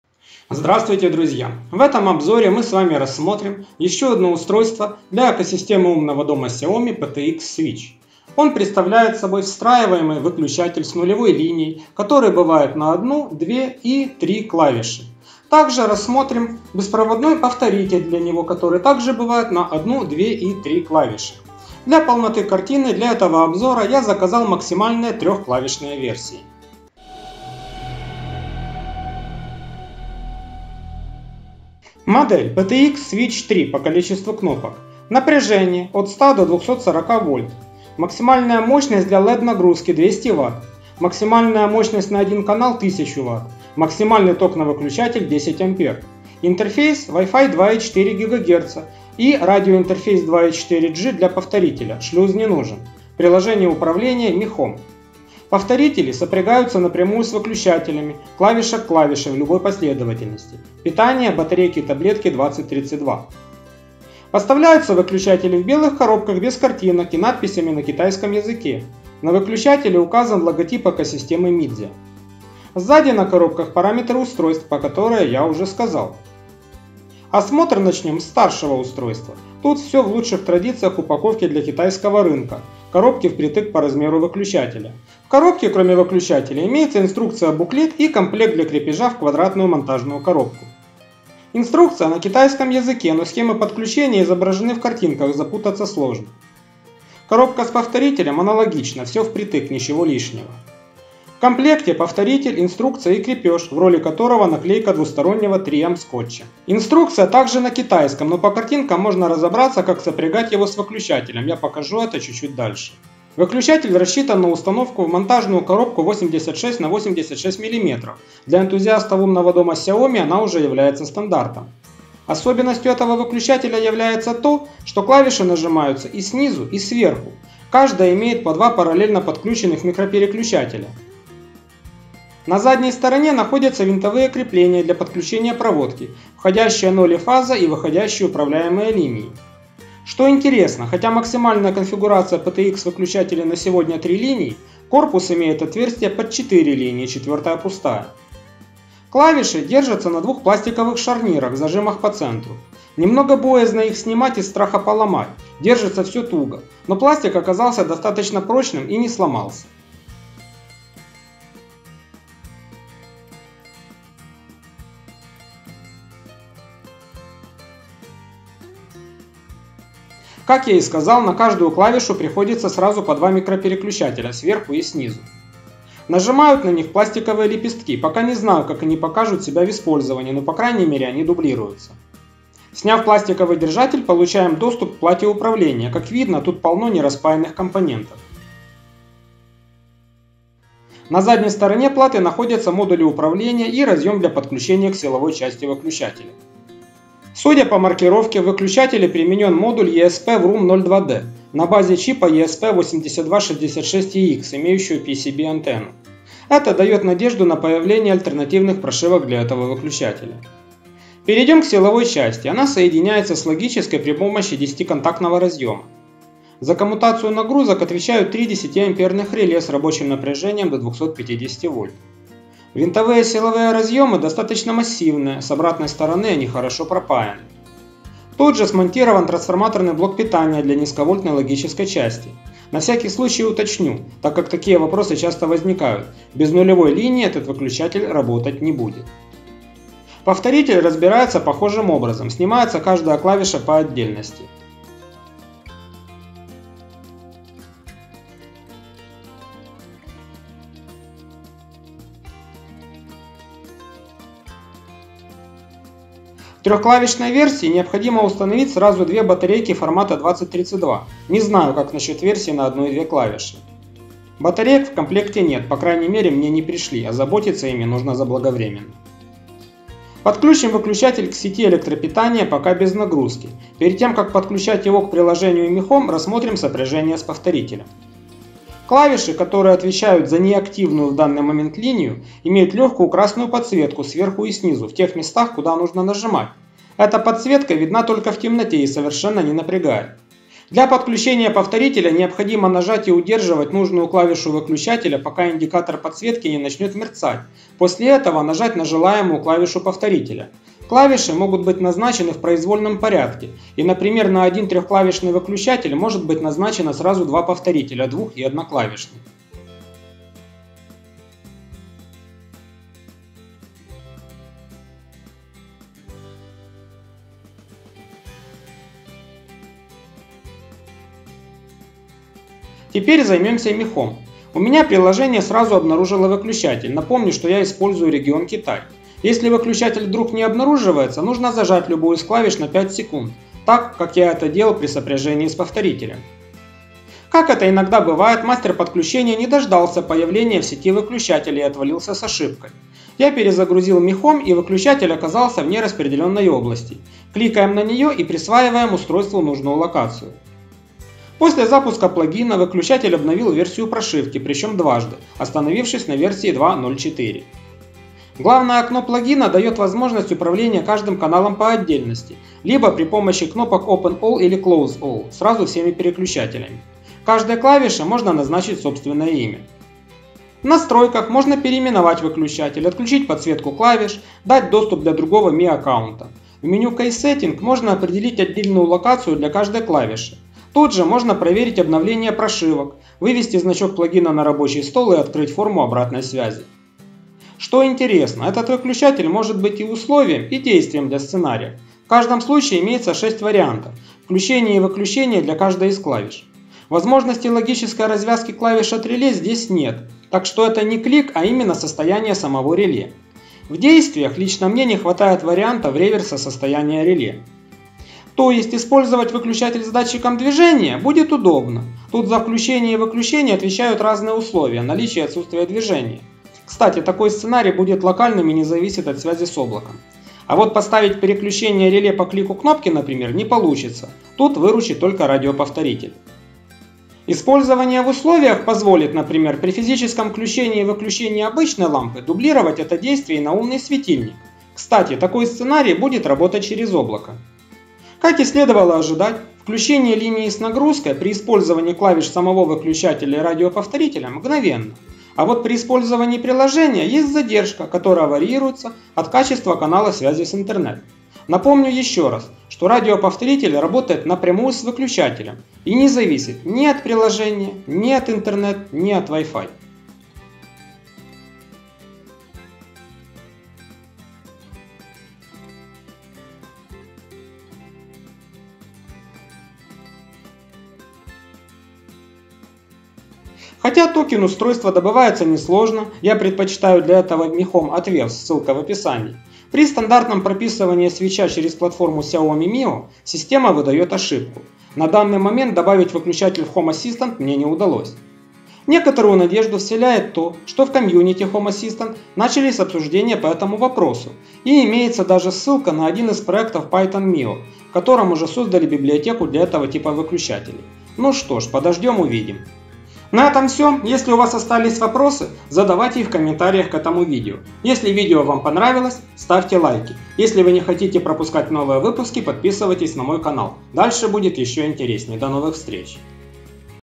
Xiaomi Mijia ptx switch - умный wi-fi выключатель с радио повторителем
В этом обзоре мы с вами рассмотрим Xiaomi - PTX switch - встраиваемый выключатель с нулевой линией на 3 клавиши,  а также беспроводный повторитель для него.

Обновление от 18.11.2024, ссылки на акутальные версии выключателей Mijia
✔️MI JIA Global Store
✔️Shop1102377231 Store